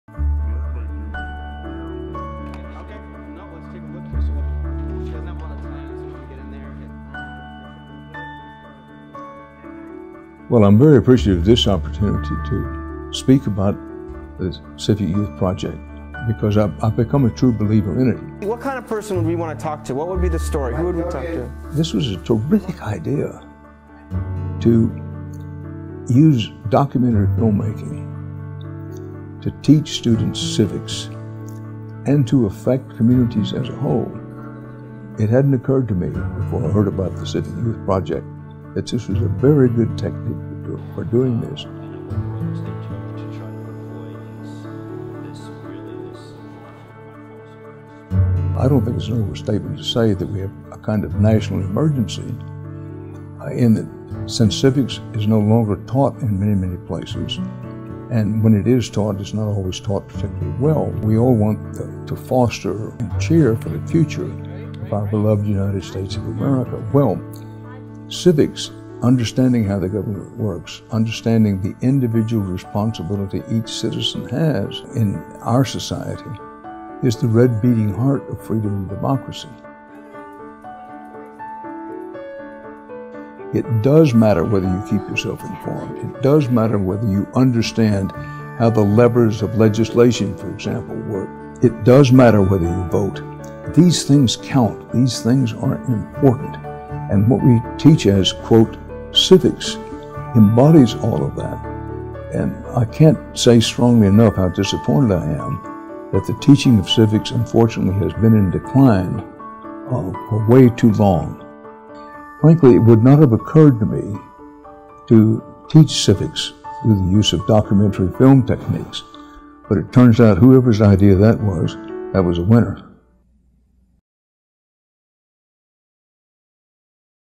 Dan Rather on Civic Life Project
The Civic Life Project uses documentary filmmaking to engage students in our democracy